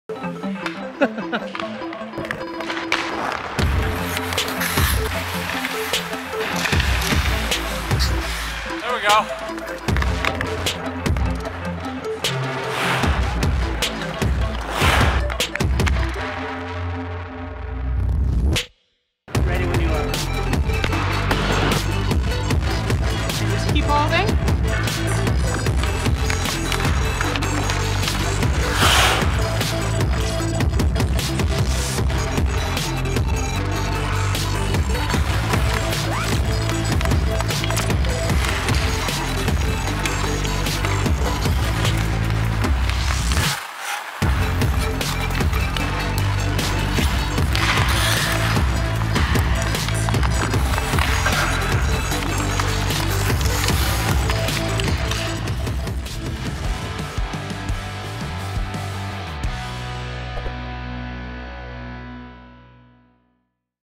Shane Wright - 2022 Upper Deck Rookie Showcase
Get an exclusive look at Upper Deck Spokesman and 2022 4th overall draft pick, Shane Wright, as he makes his rounds at Rookie Showcase.